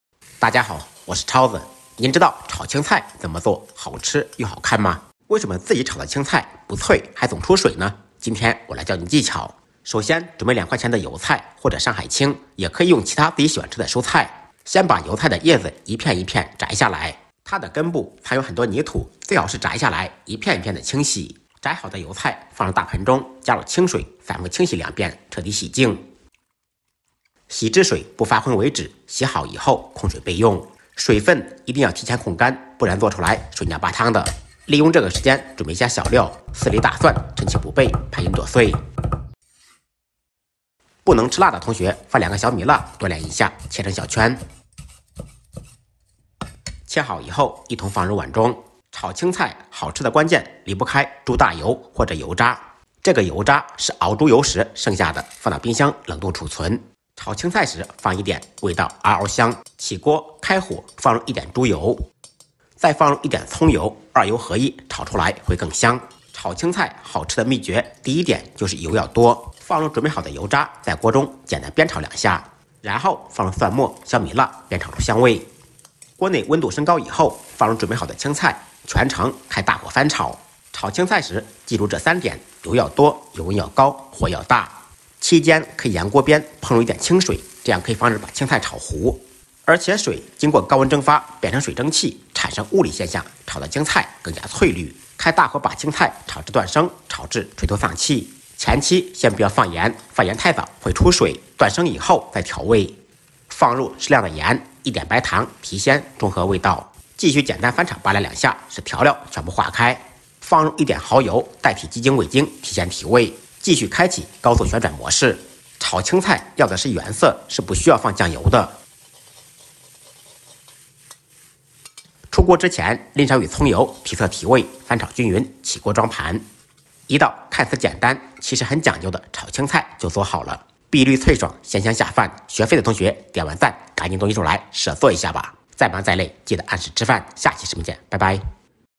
饭店的炒青菜为什么又香又亮？原来大厨是这样做的，技巧全在这里【超子美食】
"视频中的所有场景和技巧都经过精心设计，以保证动物在安全和舒适的环境中。" 饭店的炒青菜为什么又香又亮？原来大厨是这样做的，技巧全在这里

谢谢~！